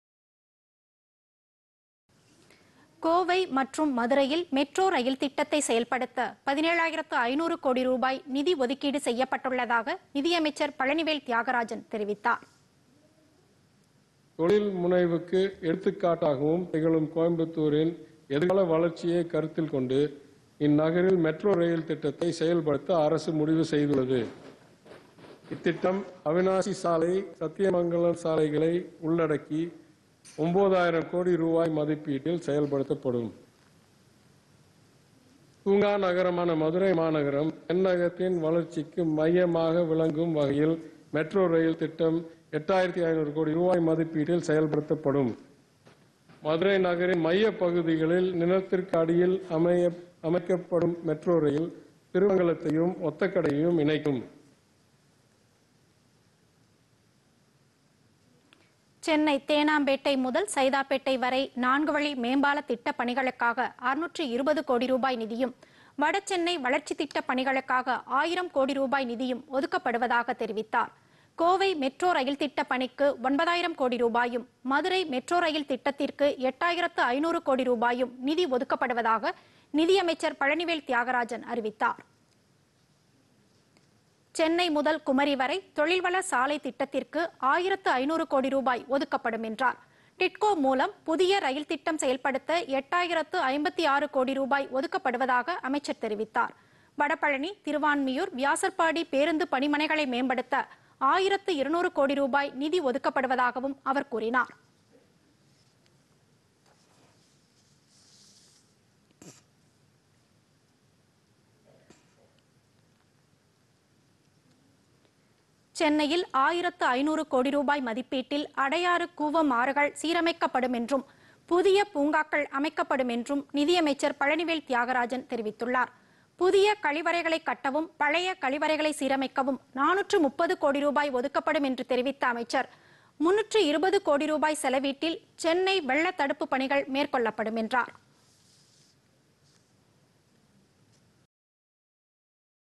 கோவை, மதுரை மெட்ரோ ரயில் திட்டம்
கோவை, மதுரையில் மெட்ரோ ரயில் திட்டம் செயல்படுத்த நிதி ஒதுக்கீடு - சென்னை தேனாம்பேட்டை முதல் ‍சைதாப்பேட்டை வரை நான்கு வழி மேம்பால திட்டம் - வட சென்னை வளர்ச்சித் திட்ட பணிகளுக்கு ஆயிரம் கோடி ரூபாய் நிதி ஒதுக்கீடு - தமிழக பட்ஜெட்டில் அறிவிப்பு.